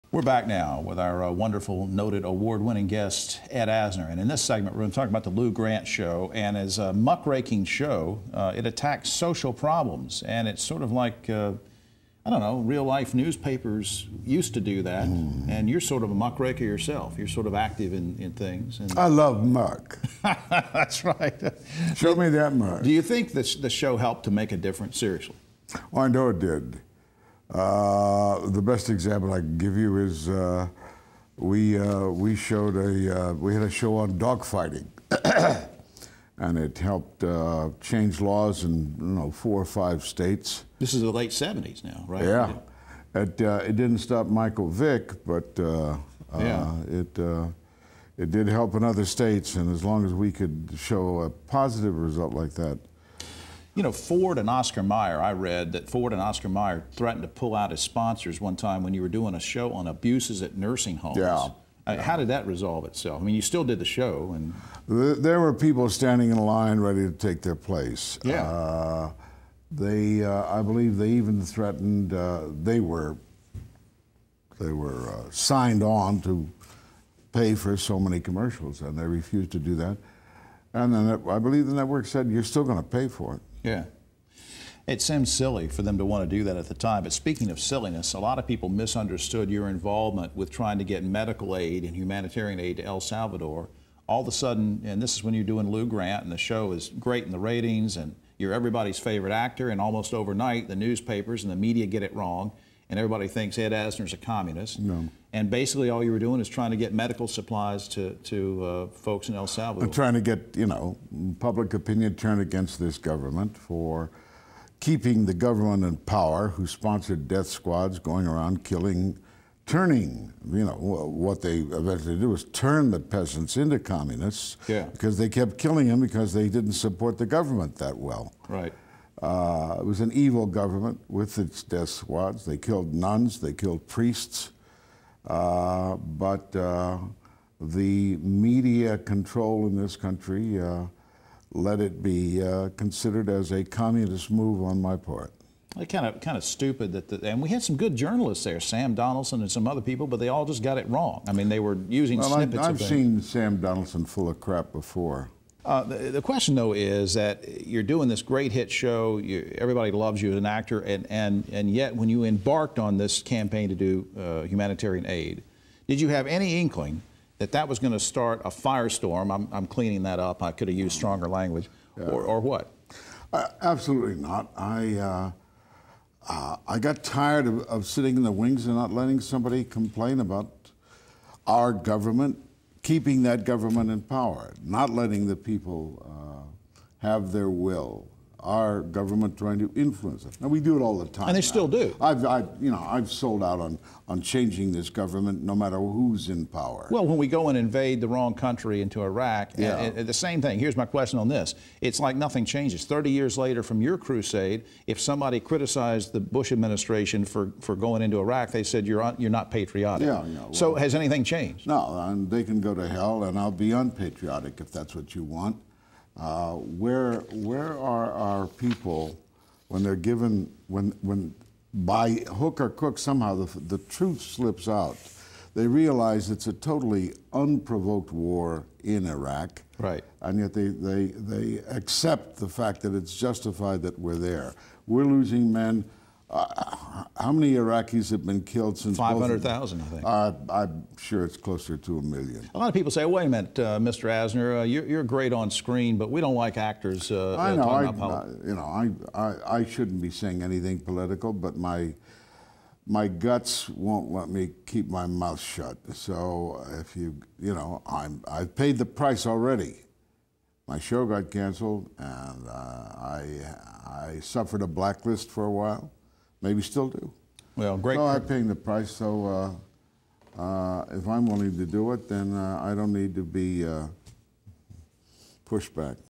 Interview with Ed Asner, part 2- January 15th, 2010
Jim Longworth interviews actor Ed Asner (The Mary Tyler Moore Show, Lou Grant)